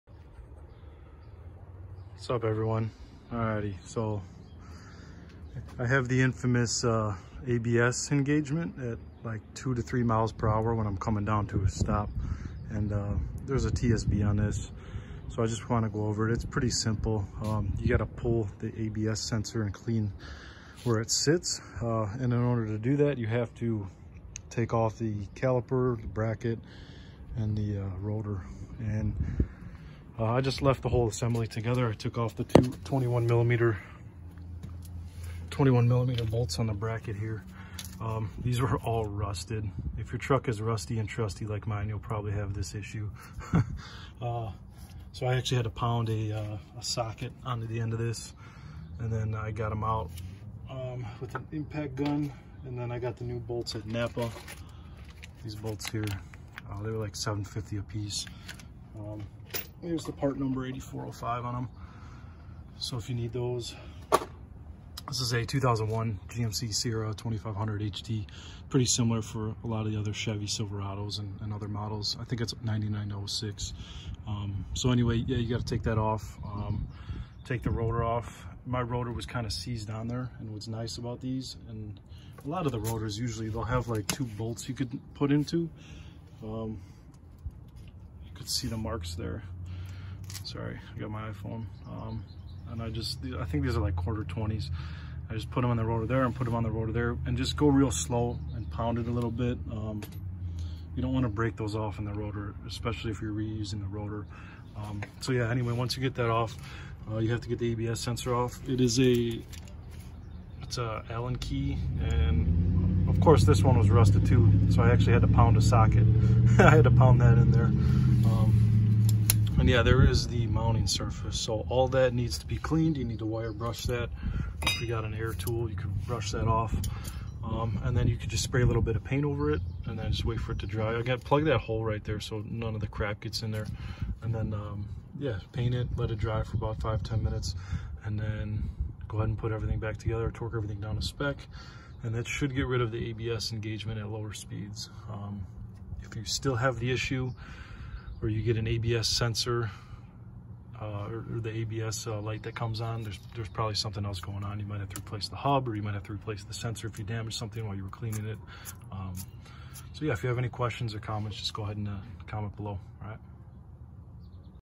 2001 GMC Sierra 2500hd ABS Engagement At Low Speed
Update: Unfortunately the sensor broke when I went to put it back in because it was so worn. Also there was play in both front hubs. So I bought 2 new sensors and hubs. Installed and no more low speed abs engagement